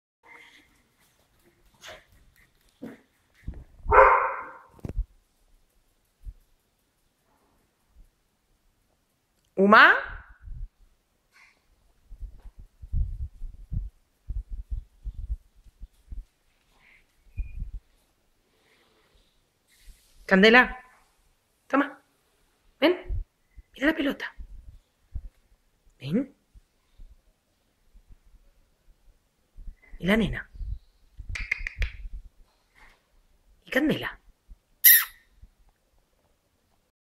BOXER CANDELA.
APARECEN TODOS MENOS UMA...  A ROBERTO NADIE LE HACE CASO EN ESTA CASA.